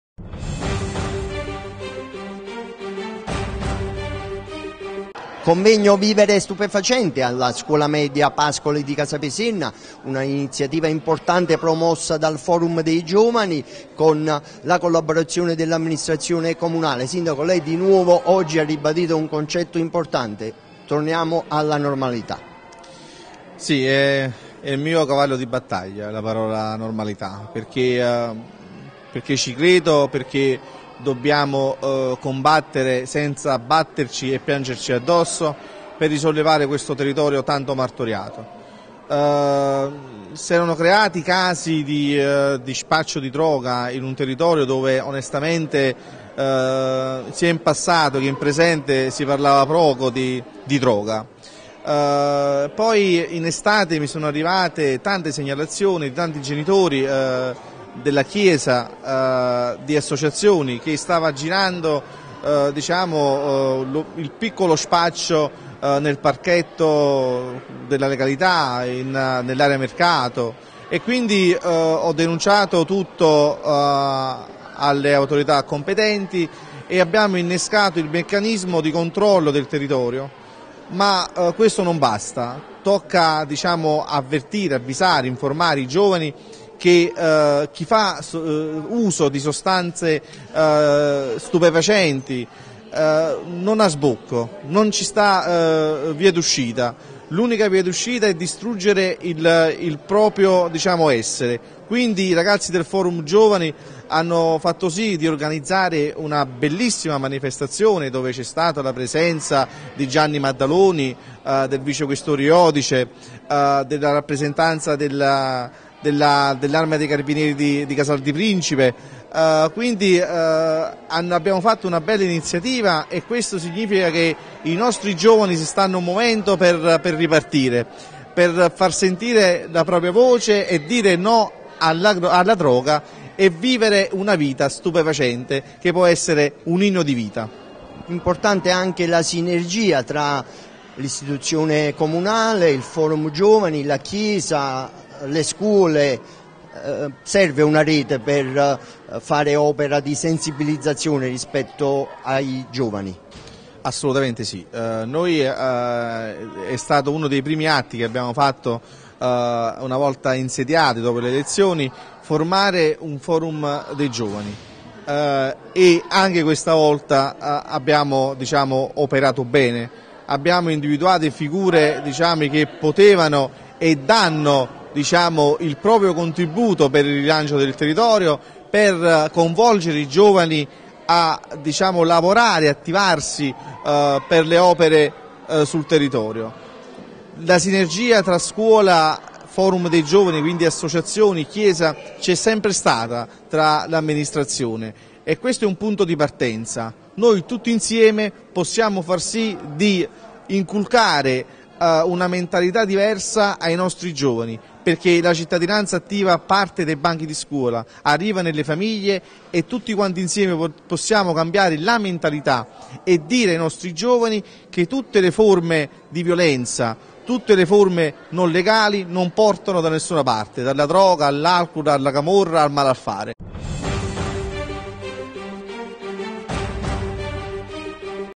Casapesenna, convegno su giovani e droga
“Vivere è stupefacente”. Questo il messaggio dell'evento che si è tenuto sabato 14 novembre presso la scuola media “G. Pascoli” di Casapesenna, su iniziativa del Forum dei giovani e dell’Amministrazione comunale per discutere di problematiche giovanili e droga.